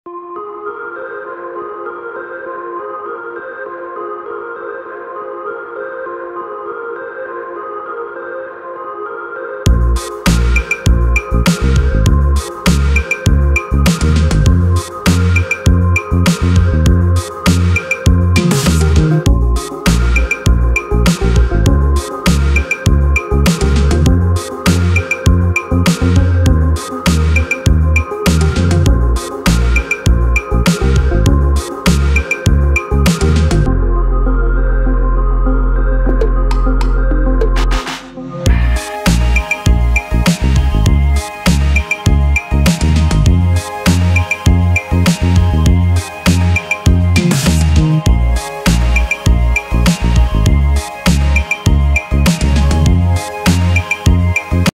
Civil Twilight Sun Rise
East Coast Sun Rise.
West Hartford, CT
Aerial videography taken with my DJ Mav 2 drone. Using my FAA UAS part 107 license.